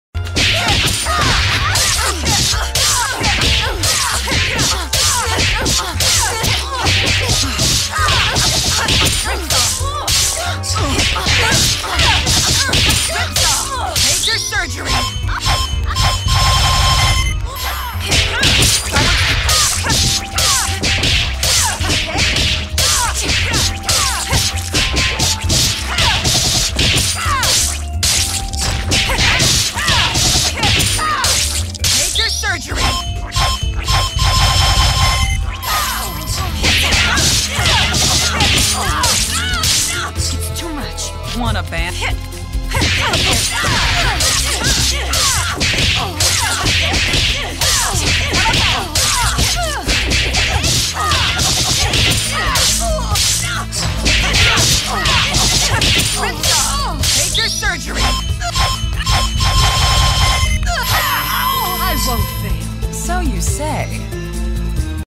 Valentine corner BnB
Three different versions, the first one works on everyone except Double, the Second one works on Middleweight and heavyweight characters, and the third one only works on Parasoul and Double. The third one is the most optimized but it is only a few hundred damage higher, the best thing about the combo is that you can adjust your height during each chain so it makes it easier to cover your mistakes and adjust your timing if you started the combo in a weird way.